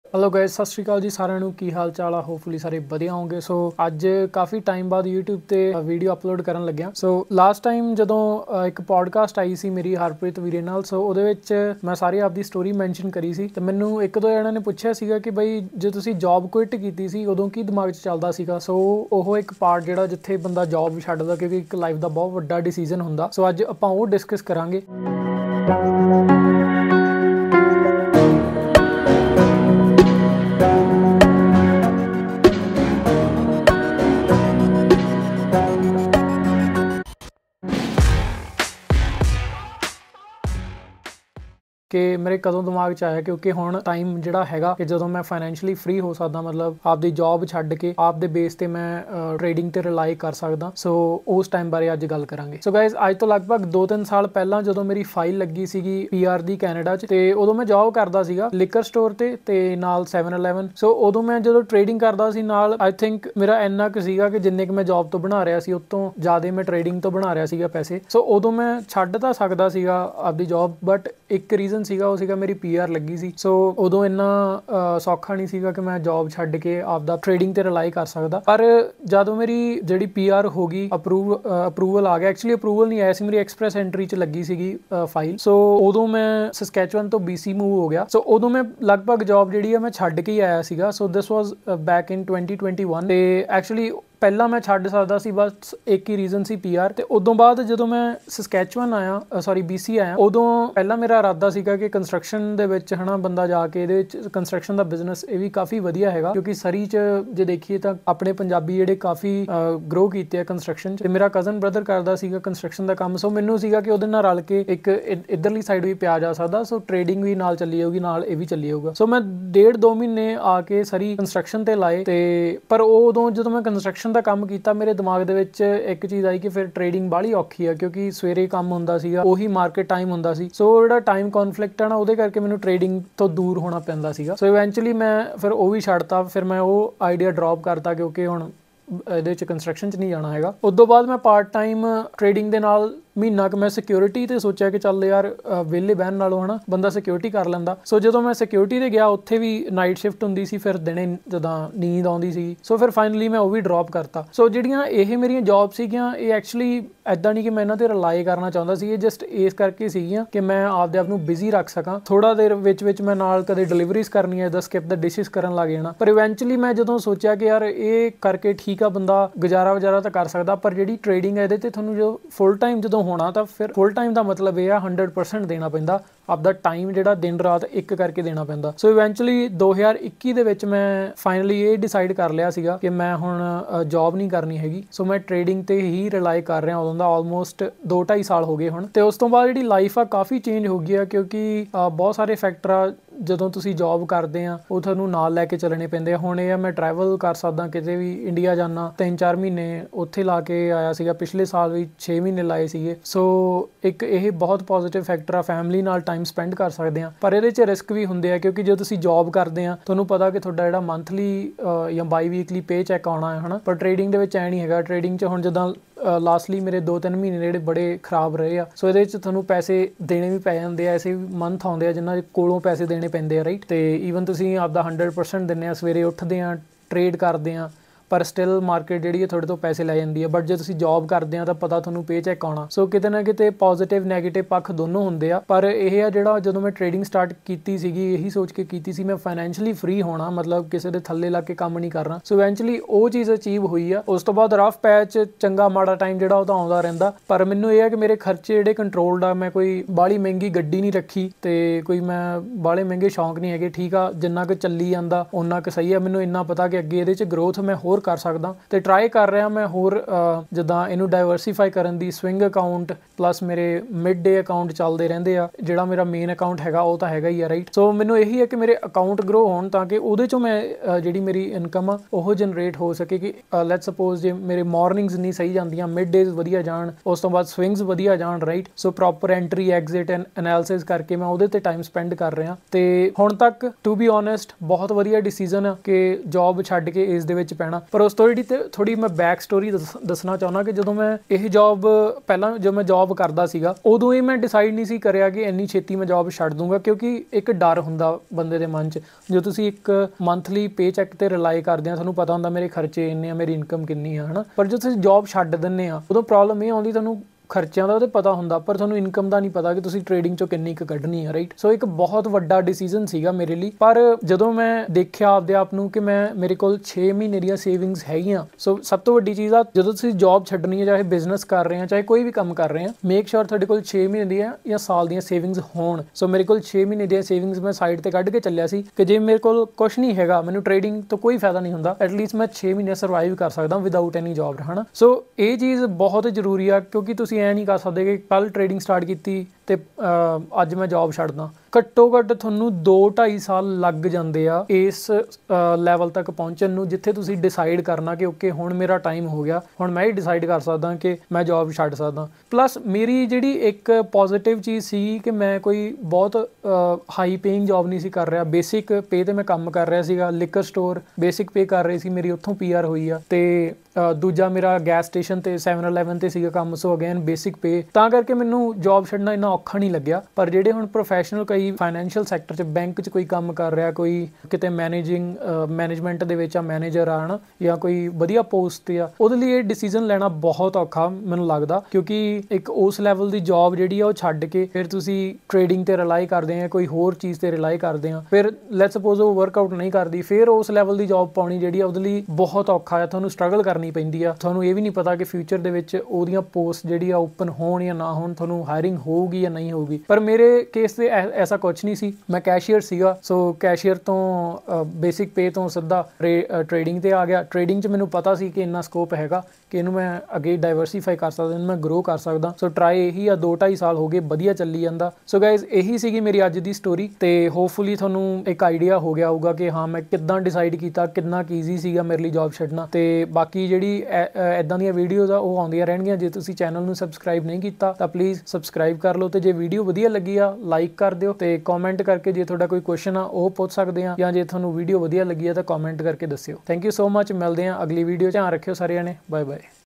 How did I QUIT MY 9-5 JOB in Canada while Trading Stocks.
In this Video i gave a brief explanation on how i was able to quit my Job in Canada in 2021. Quitting job was one of the most important decisions of my life as i was going to totally rely on Day Trading for my living. However, it has proved to be the best decision of my life as i am able to enjoy my time and travel freely anywhere i want.